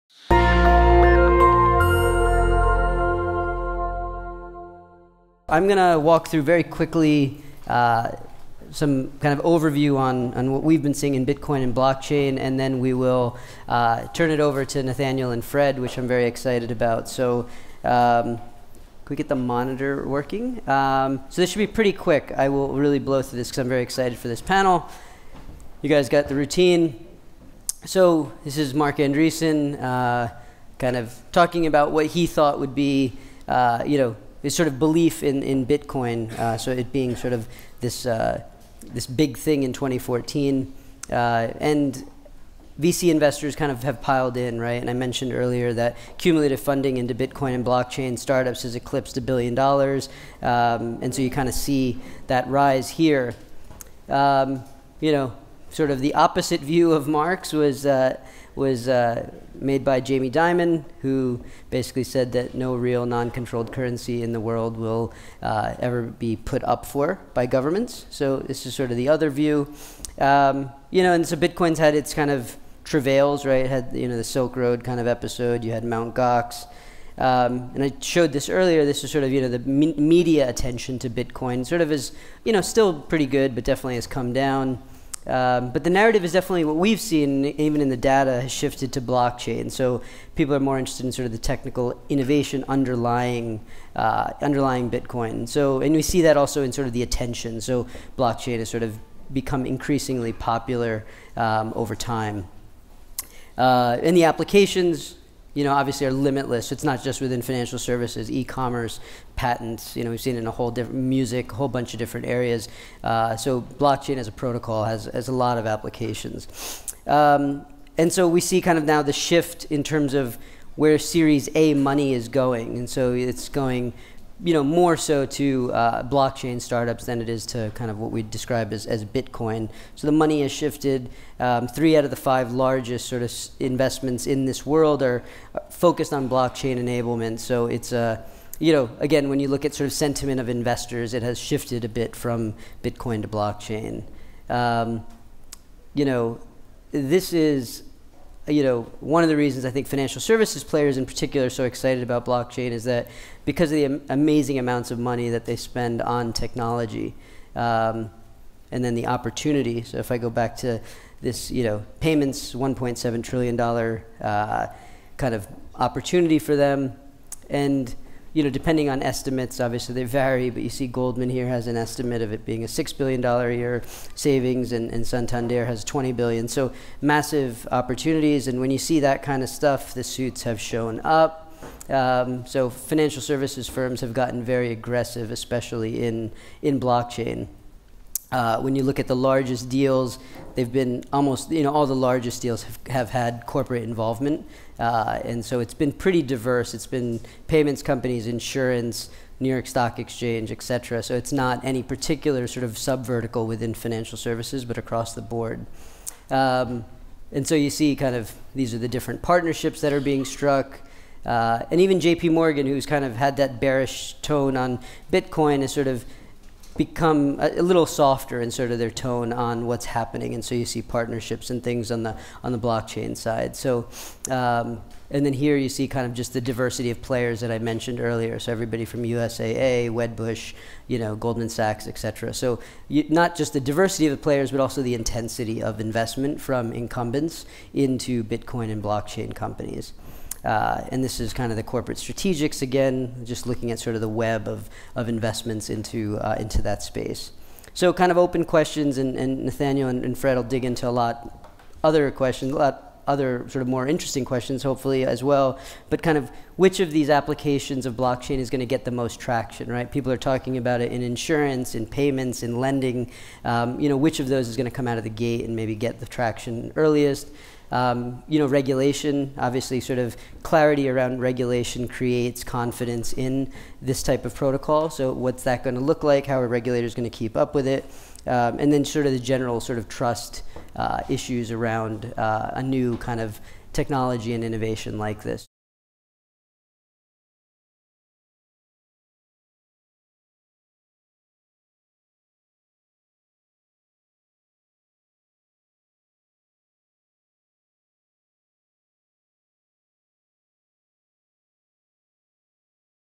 The Fall of Bitcoin and the Rise of Blockchain?
Bitcoin and blockchain are big, buzzworthy terms. Are they the next big thing or a flash in the digital pan? On stage at the Future of Fintech Conference CB Insights gives you the details on these need-to-know (but not necessarily need-to-have) technologies: distributed, non-government-linked currency and distributed ledgers that help clients know their customers. Startups are shifting away from straight-up currency and toward applications that use blockchain. $496M was invested in these companies in 2015 and finance incumbents are getting involved in the space as well.